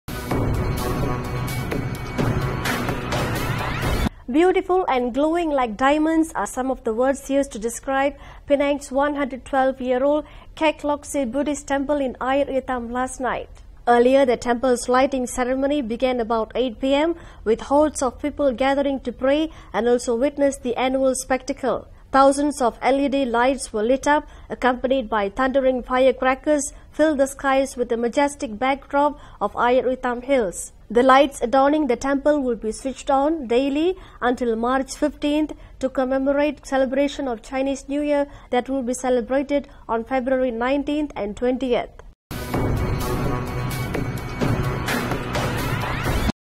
Air Itam Hills Penang Shining Bright Like A Diamond
'Beautiful' and 'glowing like diamonds' are some of the words used to describe Penang's 112-year-old Kek Lok Si Buddhist temple in Air Itam last night.

Earlier, the temple's lighting ceremony began about 8pm, with hordes of people gathering to pray and also witness the annual spectacle.
Thousands of LED lights were lit up, accompanied by thundering firecrackers filled the skies with a majestic backdrop of Air Itam hills.

The lights adorning the temple will be switched on daily until March 15th to commemorate celebration of Chinese New Year that will be celebrated on February 19th and 20th.